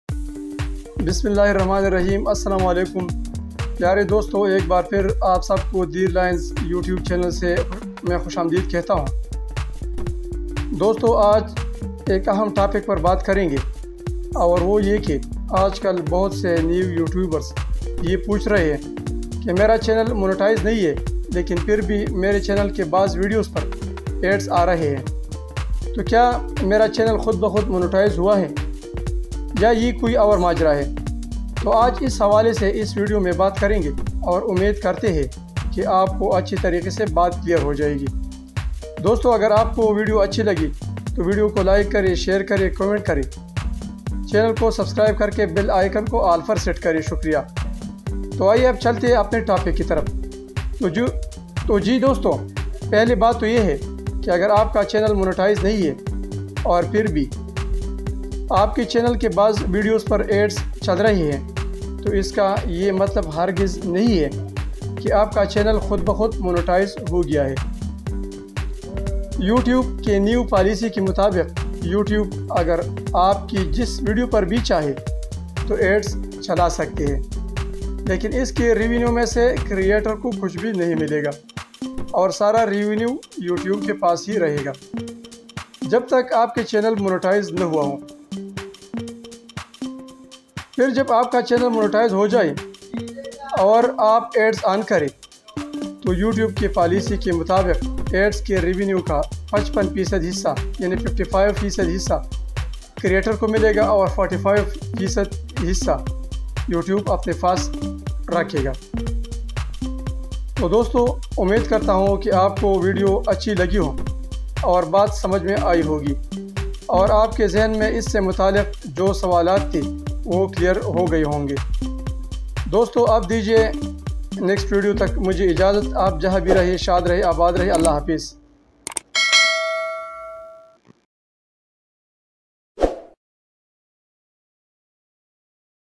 Ads on non monetize channel || Dir Lions
In this video i clear and discusse that you may watch ads on videos of non monotized  channel.